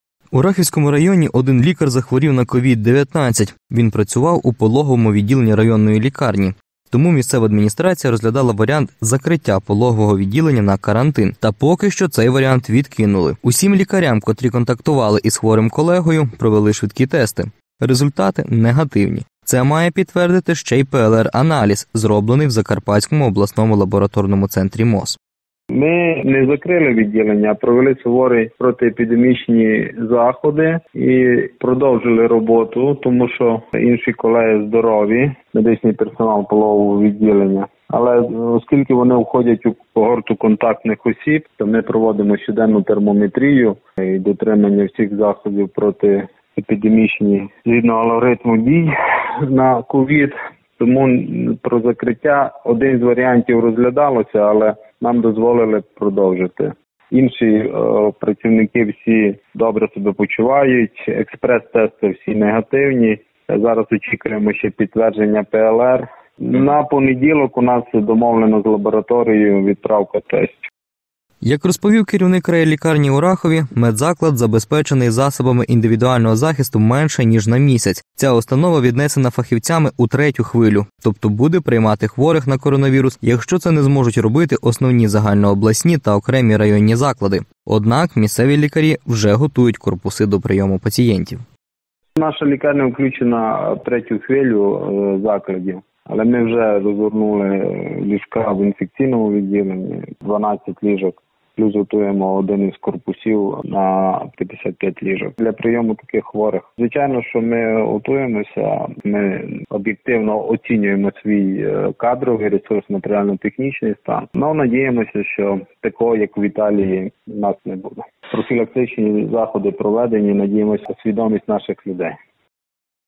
У Рахівській лікарні через хворого медика планували закрити пологове відділення. Наразі передумали
У Рахівському районі один лікар захворів на COVID-19. Він працював у пологовому відділені районної лікарні. Тому місцева адміністрація розглядала варіант закриття пологового відділення на карантин. Та поки що цей варіант відкинули. Усім лікарям, котрі контактували із хворим колегою провели швидкі тести. Результати негативні. Це має підтвердити ще й ПЛР-аналіз, зроблений в Закарпатському обласному лабораторному центрі МОЗ.